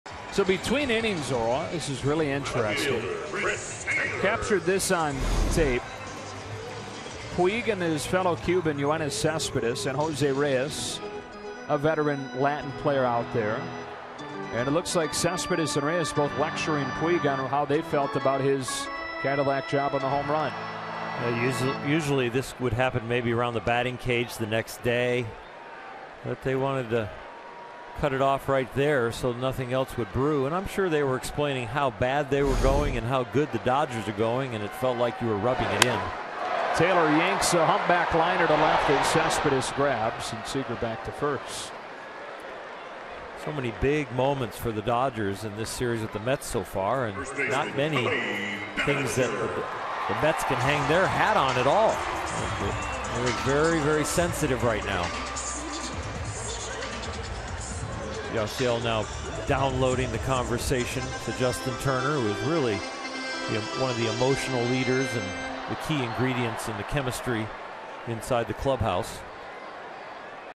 NYM@LAD: Puig talks to Cespedes, Reyes between frames
Yasiel Puig has a discussion with both Yoenis Cespedes and Jose Reyes following his earlier exchanges with Wilmer Flores and Travis d'Arnaud